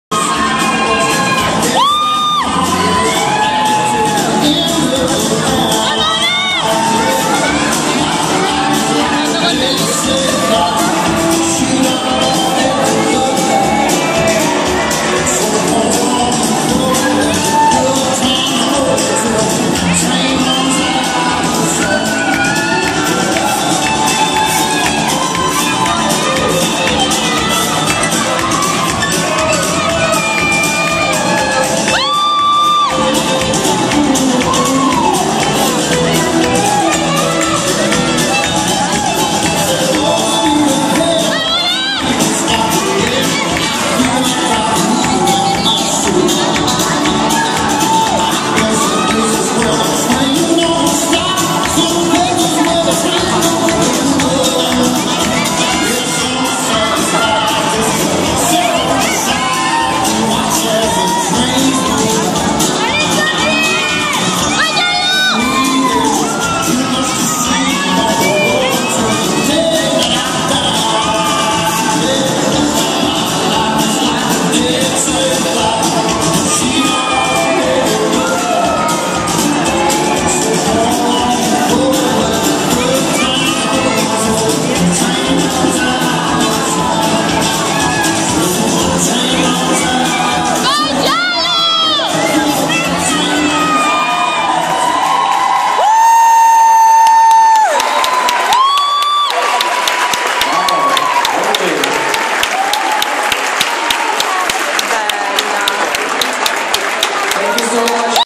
WCDF ITALY 2013 - NOVICE ADULT M - LILT
WCDF ITALIAN CHAMPIONSHIP 2013

22/24 novembre 2013 - Milano (IT)
1° Classificato NOVICE ADULT M: Gianluca
1° Classificata NEWCOMER CRYSTAL F: Laura Reina

Sicania West Line Dance Academy